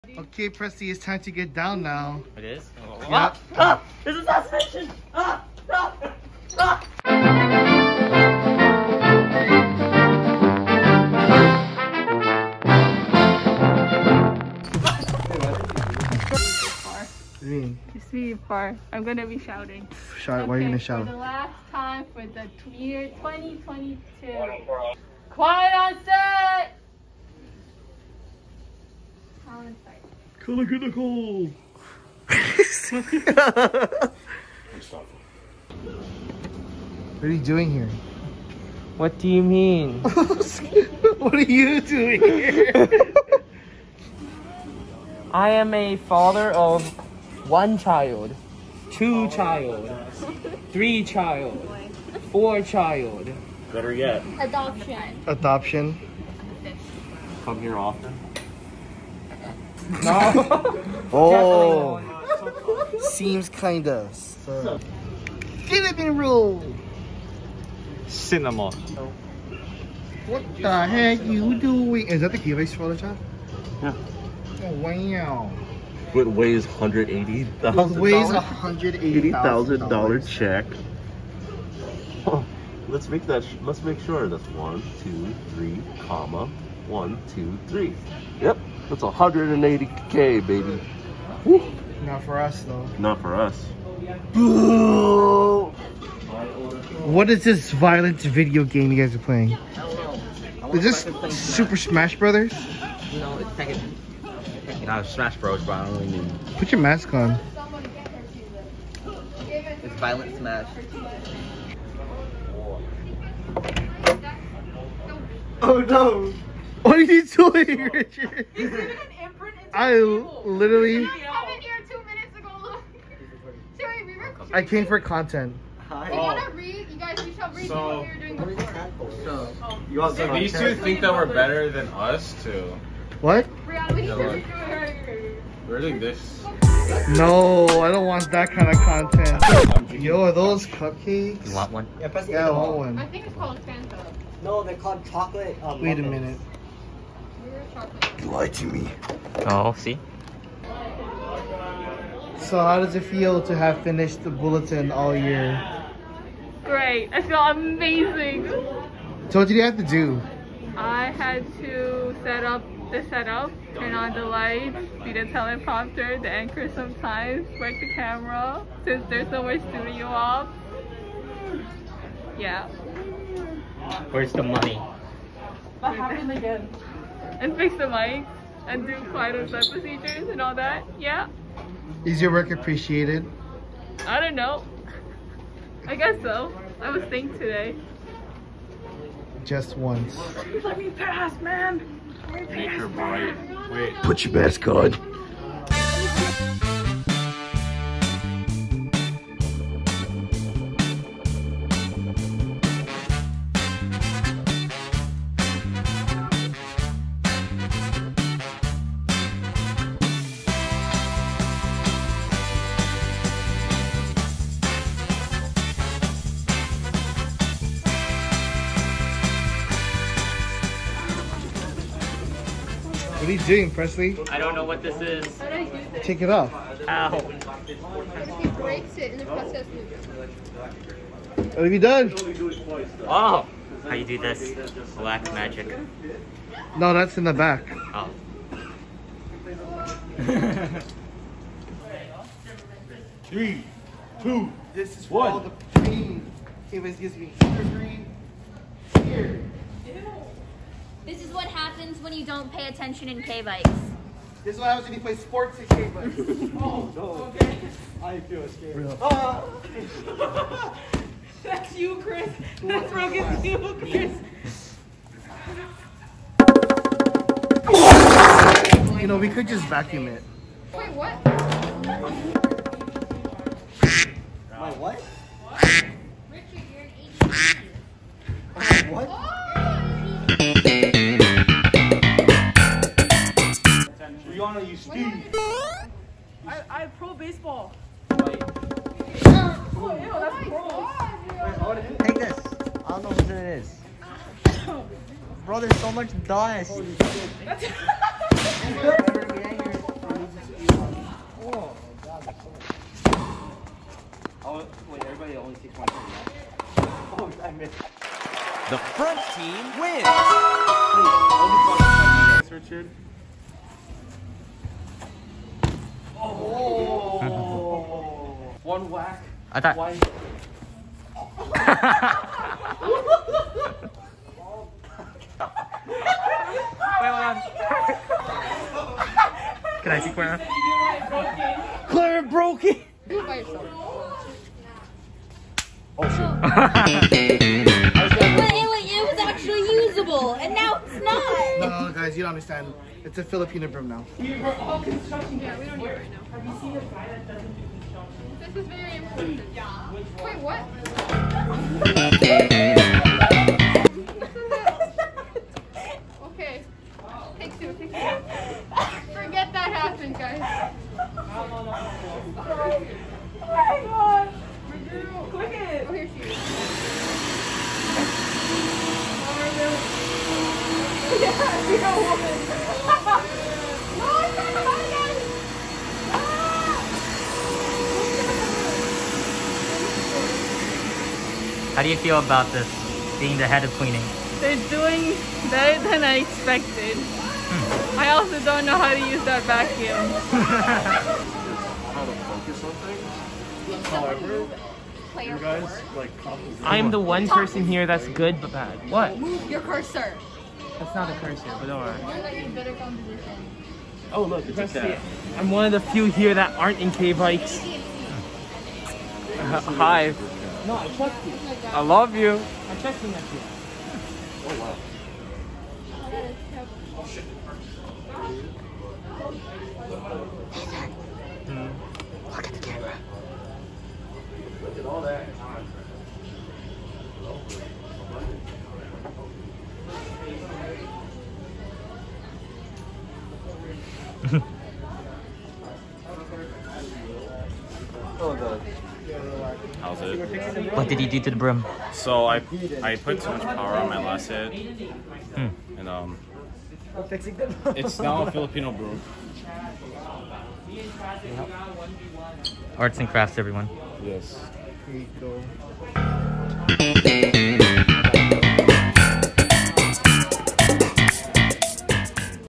The Dust Bowl | KVIKS Office Hours
Our annual end of year cleaning always ends in disaster. Here's the story of the broken broom.